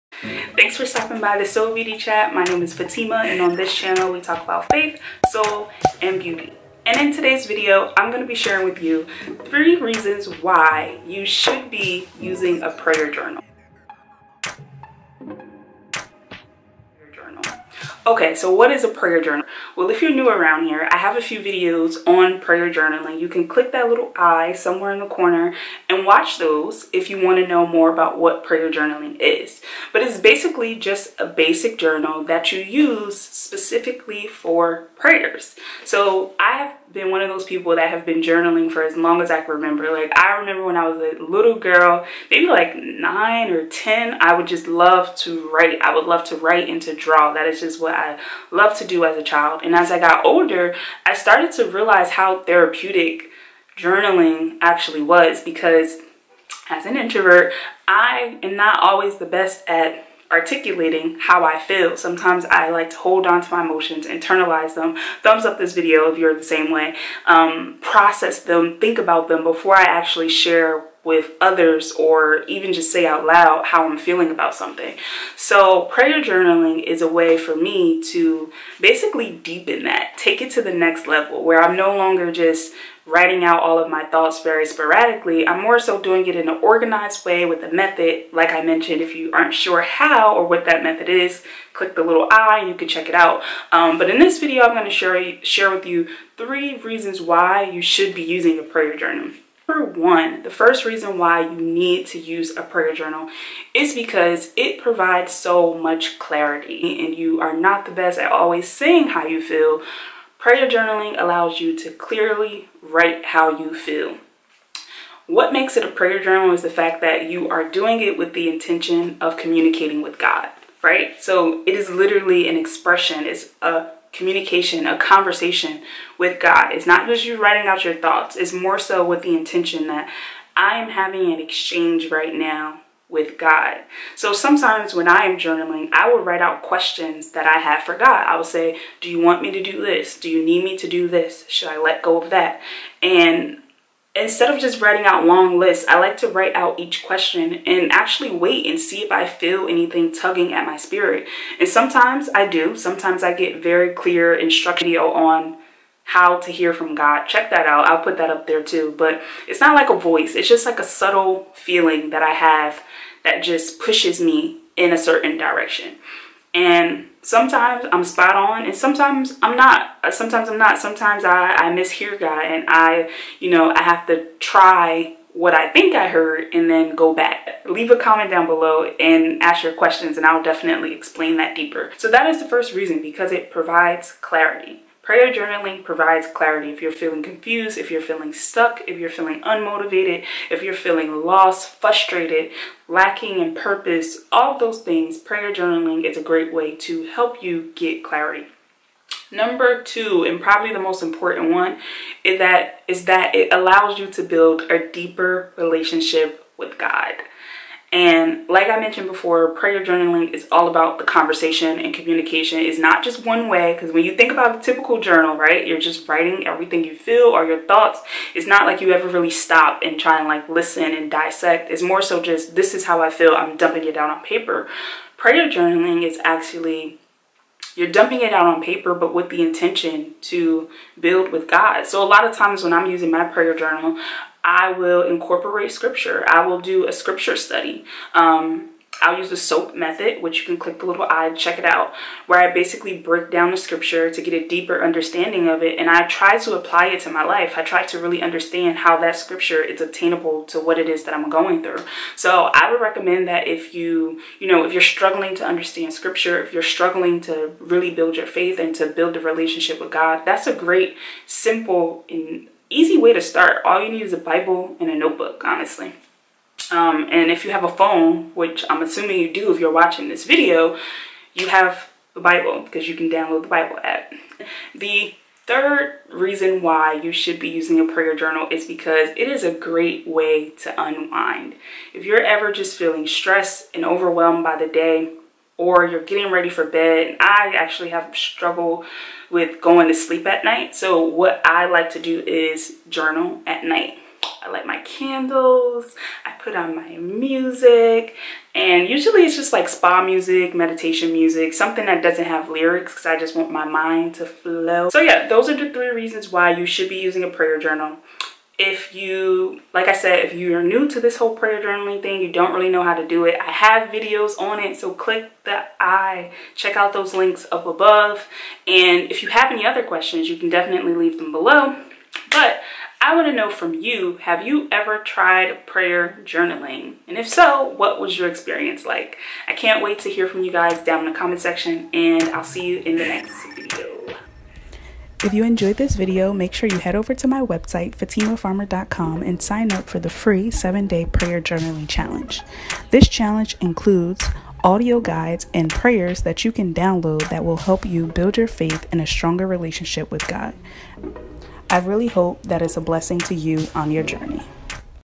PRAYER JOURNALING FOR BEGINNERS | 3 REASONS YOU SHOULD BE USING A PRAYER JOURNAL 📖🙏🏾
AMAZON STOREFRONT(decor, hair, & more)